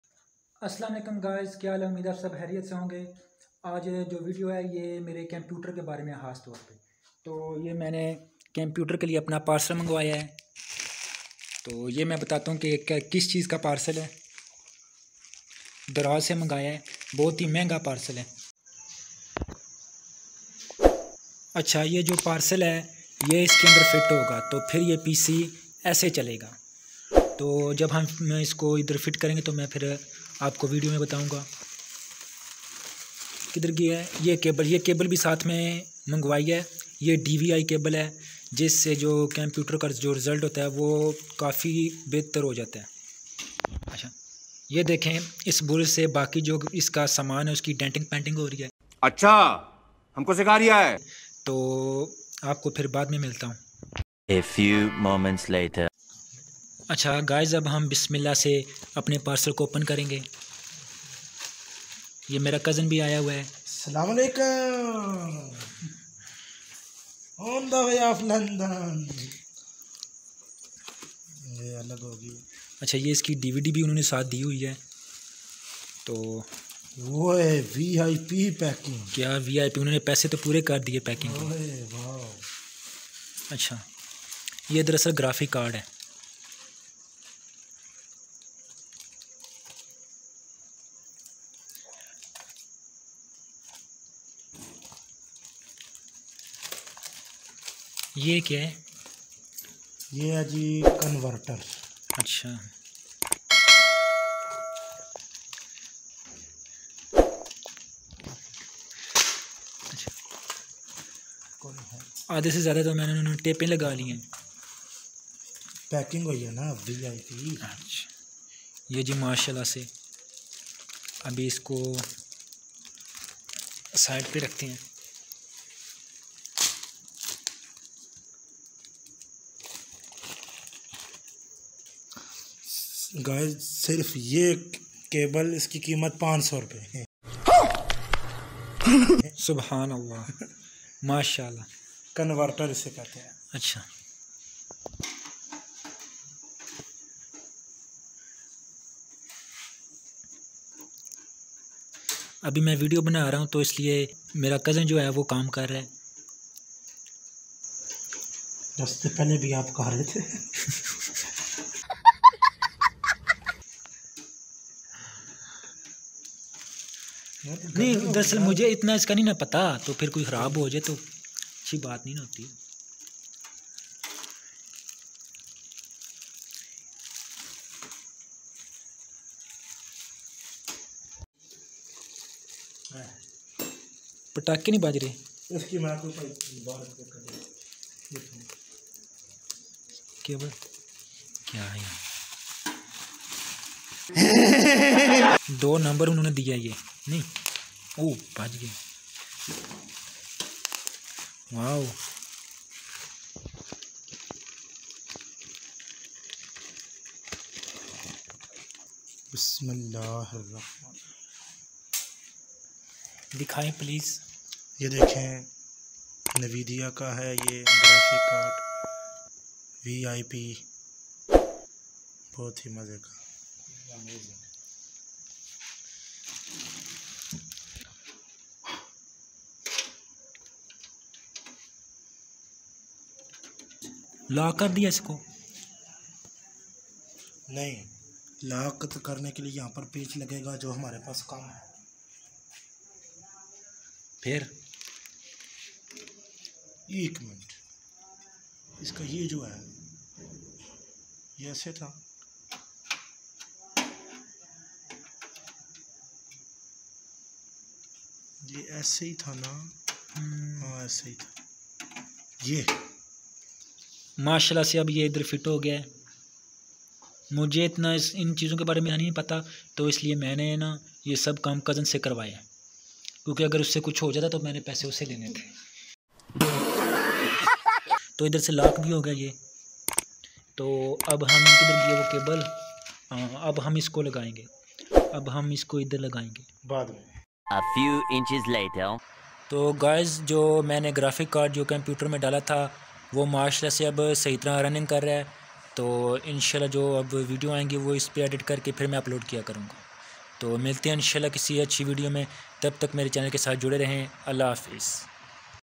Pc m Graphic card NVIDIA ko set kia .Best Vlog|| Daraz se parsal mangwaya ..
Daraz se parsal ko pc m fit kia. itna manga parsal.. 😱😱

best vedio pc